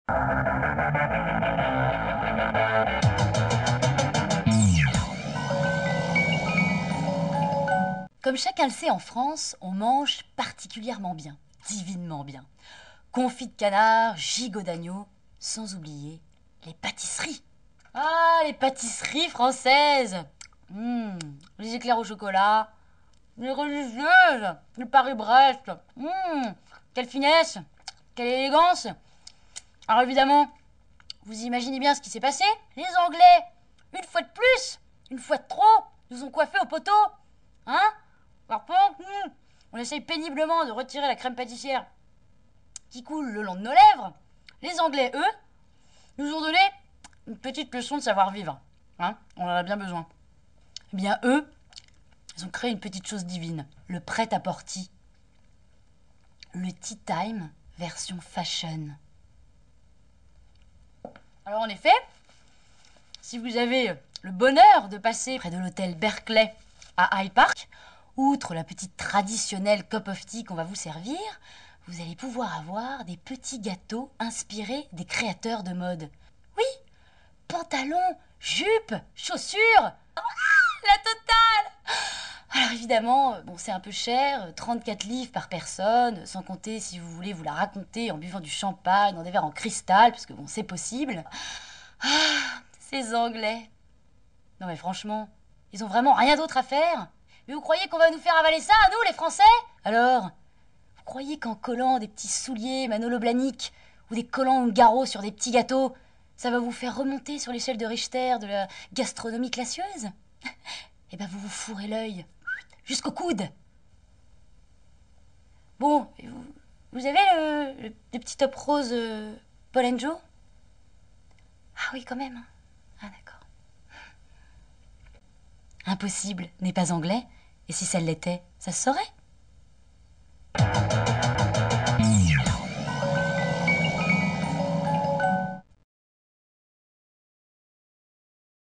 Découverte du prêt à portea à Londres
Découverte du prêt à portea à Londres, une pause thé avec des gâteaux imaginés par les plus grands créateurs de mode. By newlondon.fr